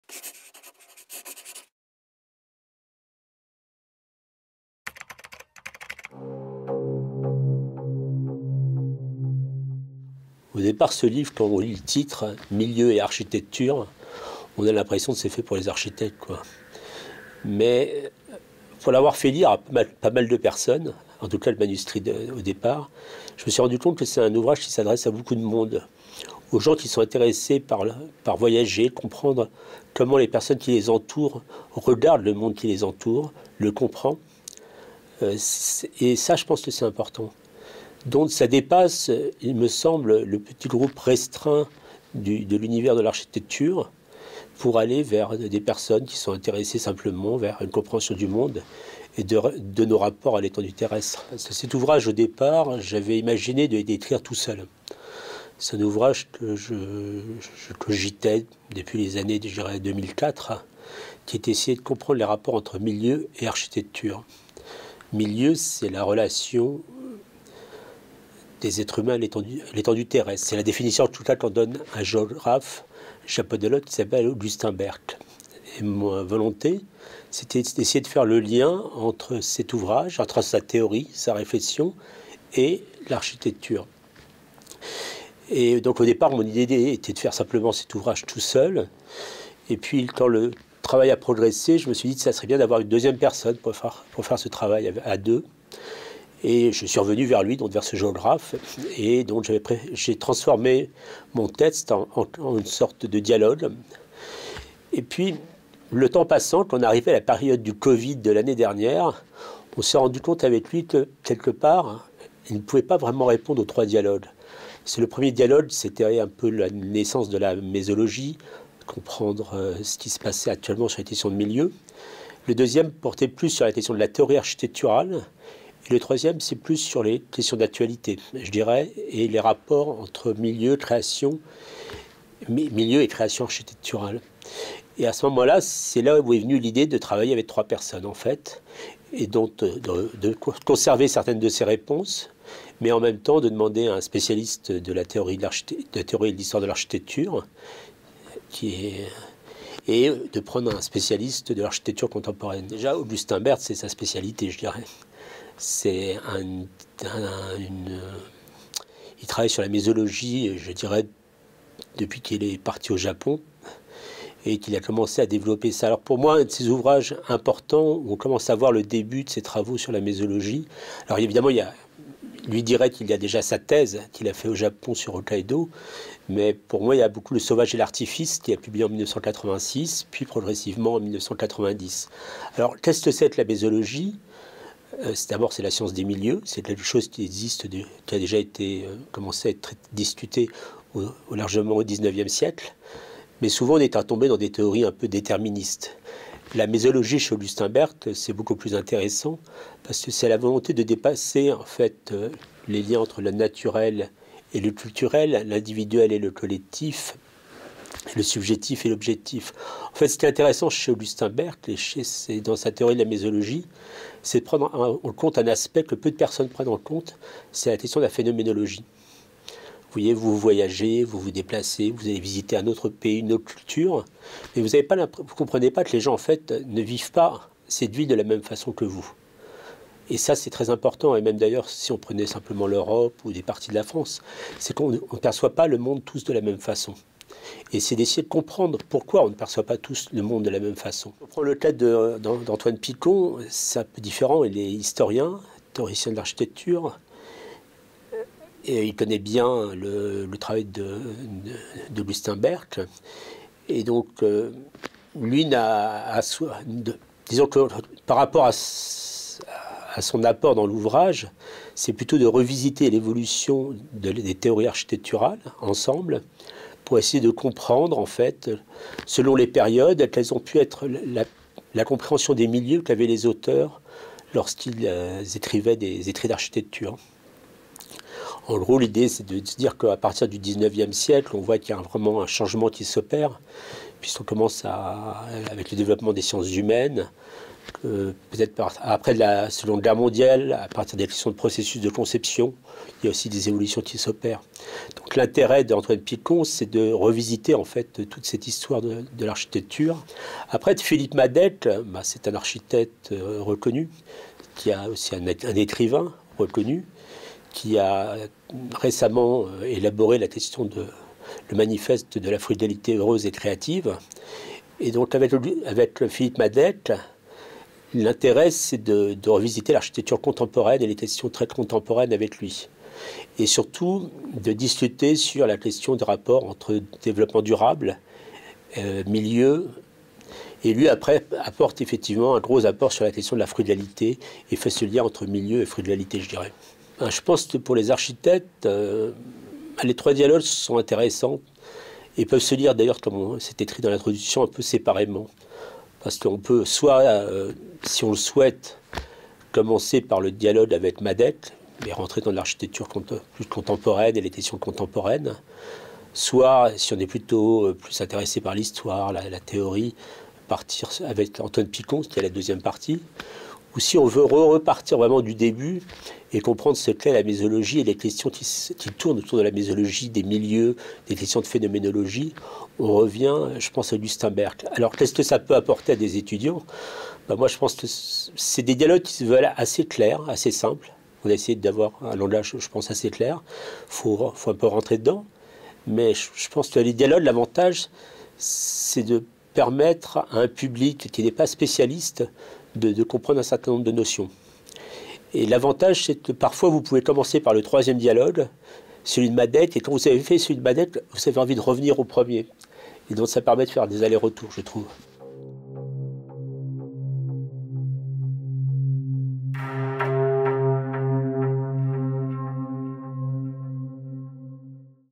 Yann Nussaume - Milieu et architecture
Yann Nussaume vous présente son ouvrage "Milieu et architecture : entretiens avec Augustin Berque, Philippe Madec et Antoine Picon" aux éditions Hermann.